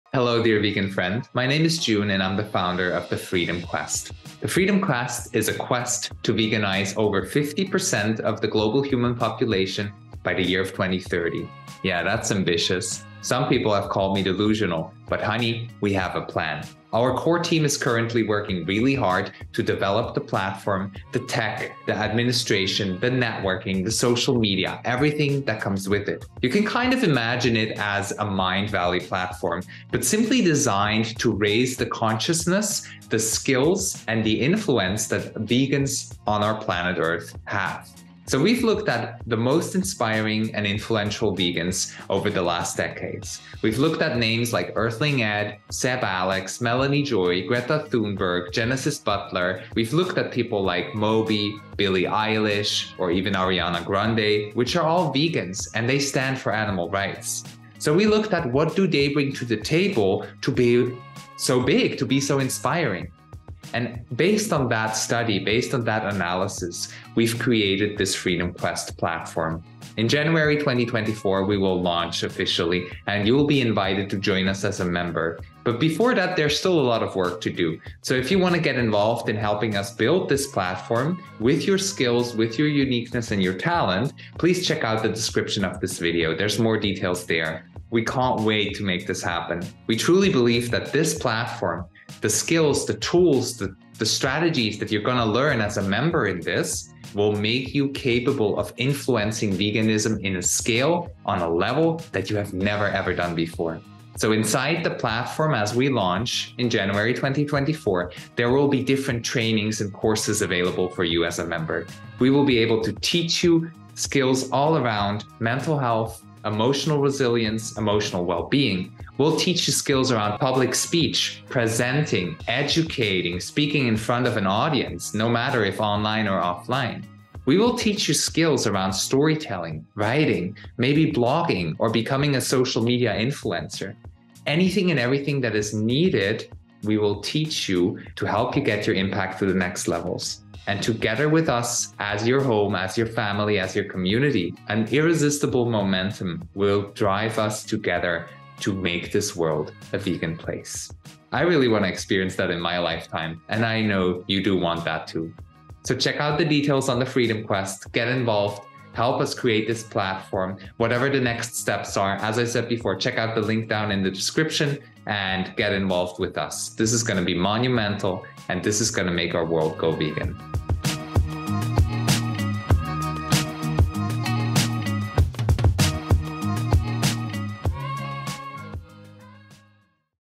Attention Vegans! 🌱 Introducing The Vreedom! Quest
Imagine it’s 2030 and over 50% percent of people on the planet are Vegan! We look back at a time where eating animals and consuming animal products was normal.

We’ve grown into the majority of people on this planet and with that saved and freed billions of animals from the cruelty they used to experience. On top of that the environment recovered itself faster than ever and humans feel healthier & happier than ever before!

Approximately 79 million people on the planet are currently Vegan. That's only 1.1% of all people on earth!

One decade ago it was less than 0.5%. That means compassion and love for animals & the planet has grown a bit. Is that enough to create a world free from the harm and suffering caused to our beloved animal friends? No it's not! And that's why we Vegans have to step up the game and create more impact.

My Name is Jun Fuchs and I am the founder of the "Vreedom! Quest" platform. Learn more about the platform, our vision & purpose ⬇️

Our platform is designed to help Vegans worldwide elevate their impact & influence. We offer high-quality trainings, courses, and an irresistible community.

We plan the official launch of the "Vreedom! Quest" platform in January 2024.

As we are creating the foundations of the platform, we currently look for new core-team members with specific skills and a undimmable flame for animal rights! Want to get involved?